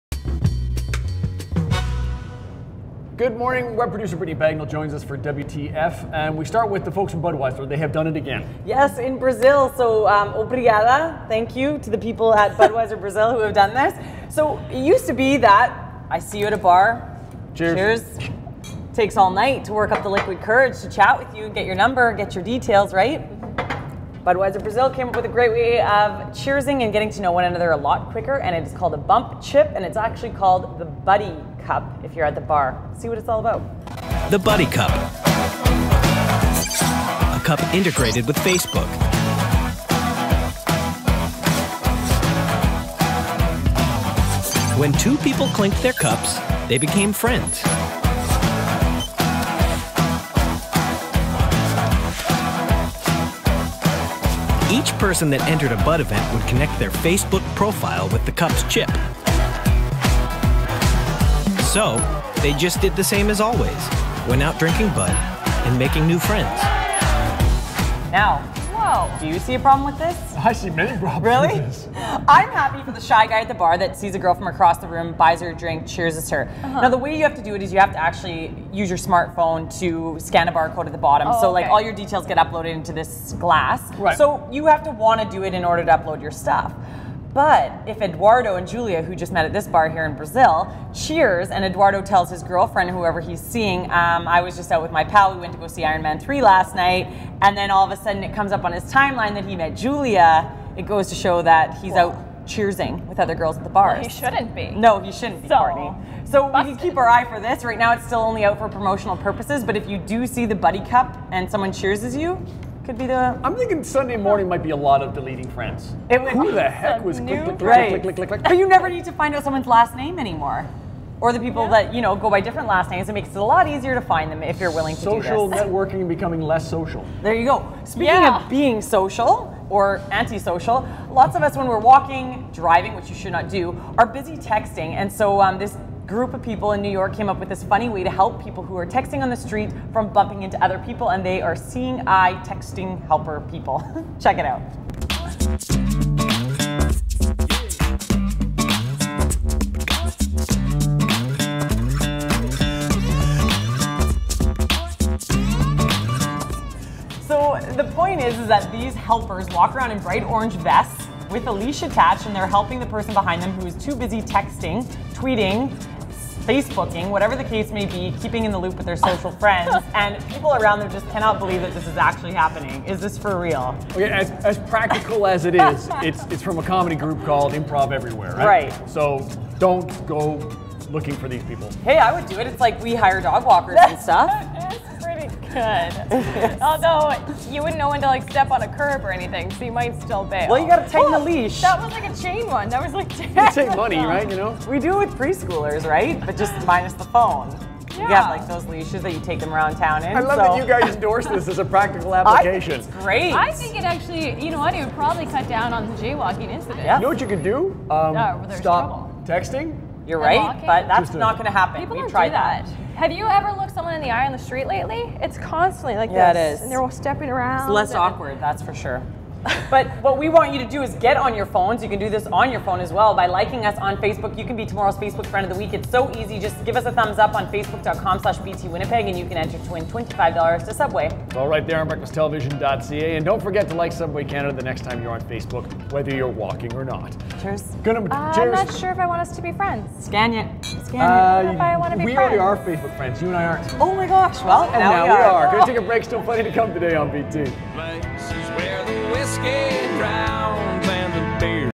WTF: Buddy Cup & Seeing Eye People
Brittany dishes on an easier way to find friends on Facebook and how texting while walking can be made safe in NYC